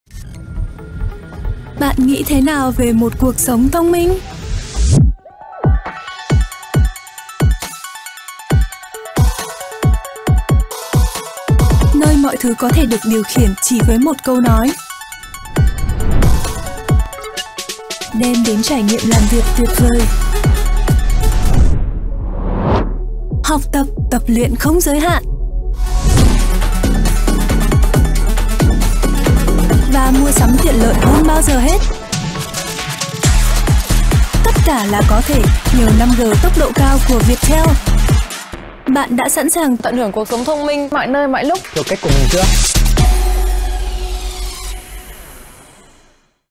5G VIETTEL CUỘC SỐNG MỚI
Mạng 5G Viettel có vùng phủ sóng 63/63 thủ phủ tỉnh/thành phố toàn quốc ngay tại thời điểm khai trương.
Các giải pháp về kỹ thuật công nghệ cho mạng 5G được Viettel triển khai là hiện đại nhất, đang được các nhà mạng hàng đầu thế giới sử dụng.
Viettel cung cấp 11 gói cước 5G trả trước và 8 gói cước trả sau, dung lượng gấp 2 so với gói 4G tương đương, đi kèm theo kho nội dung cao cấp tại đây